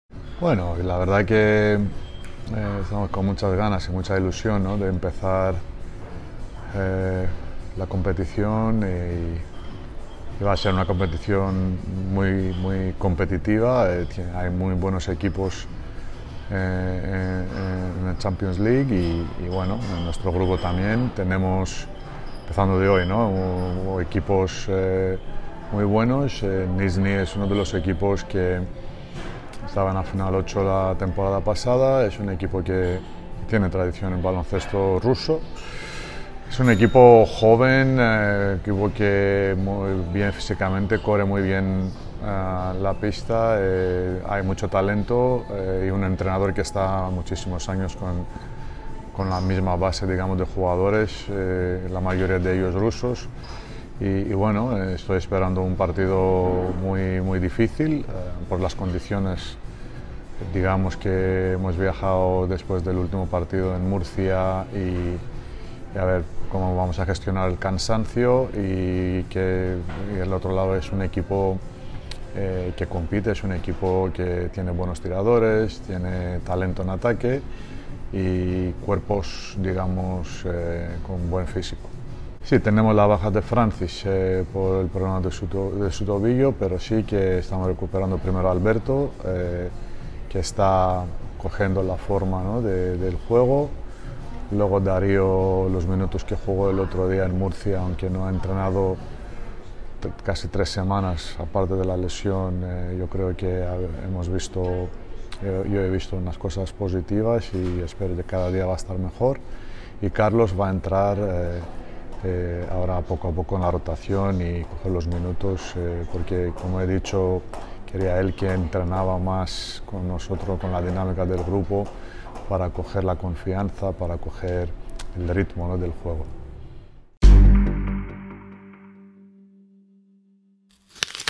Katsikaris: "Ganas e ilusión por empezar la BCL"
El entrenador del Unicaja Fotis Katsikaris analiza el primer partido del equipo este miércoles en la Basketball Champions League (BCL), competición en la que jugará por primera vez en su historia. Además, valora al rival, el Nizhny-Nóvgorod (18:00 horas, retransmitido por Andalucía TV, DAZN). Los aficionados podrán verlo en Le Grand Café Centro con la retransmisión in situ de Unicaja Baloncesto Radio